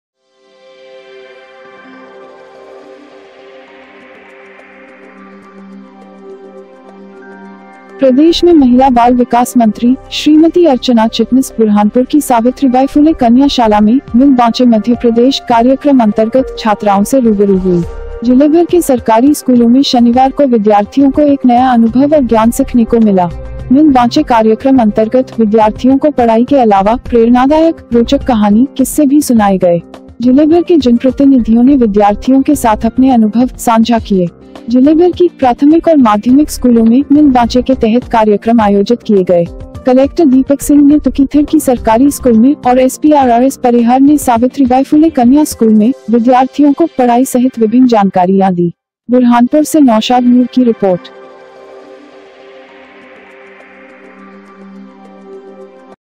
प्रदेश में महिला बाल विकास मंत्री ने स्कूली बच्चों को पाठ पढाया - MP NEWS NETWORK BURHANPUR
प्रदेश में महिला बाल विकास मंत्री श्रीमती अर्चना चिटनिस बुरहानपुर की सावित्रीबाई फुले कन्या शाला में  मिल बांचे मध्यप्रदेश  कार्यक्रम अंतर्गत   छात्राओें से रूबरू हुई।